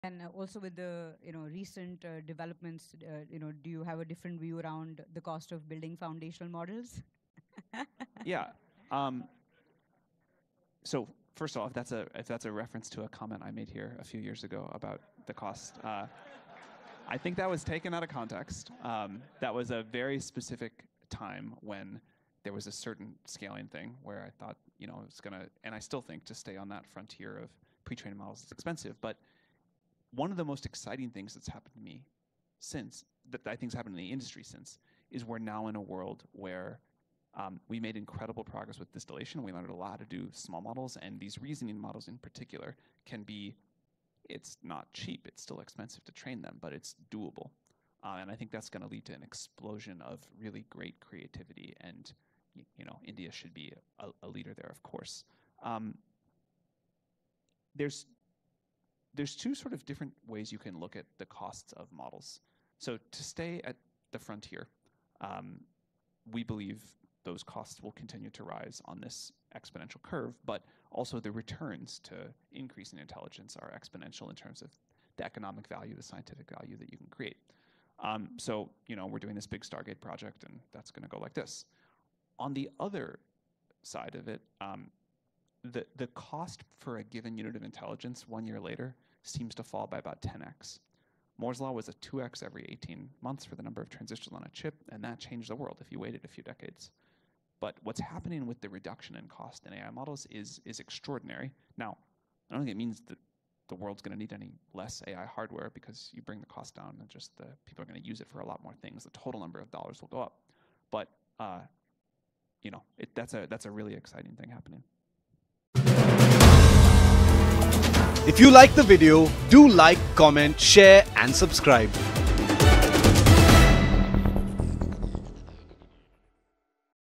AI Cost Revolution: Sam Altman On Making Intelligence 10X Cheaper!
In this revealing discussion, Sam Altman, CEO of OpenAI, revisits his earlier remarks on AI model costs and provides fresh insights into the evolving economics of foundational models. Altman explains that while staying on the cutting edge of pre-trained models remains expensive, breakthroughs like model distillation and efficient reasoning models are transforming the landscape. He highlights a dual trend: the cost to develop state-of-the-art models is rising exponentially, yet the cost per unit of intelligence is dropping by roughly 10x each year—a pace that far outstrips Moore's Law. This dramatic reduction is expected to fuel an explosion of creativity and innovation worldwide, with emerging markets like India poised to play a leading role. Altman also hints at ambitious projects like OpenAI’s "Stargate" initiative, underscoring that while hardware spending may continue to grow, the economic and scientific returns on AI advancements are set to skyrocket.